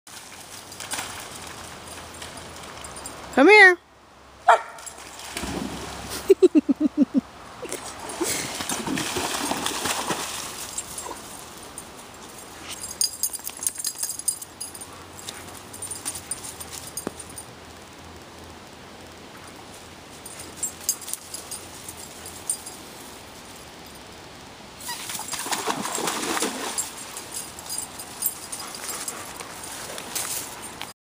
Bolt in the back yard
s enjoying his last week in the back yard bcfore it snows and bfore we leave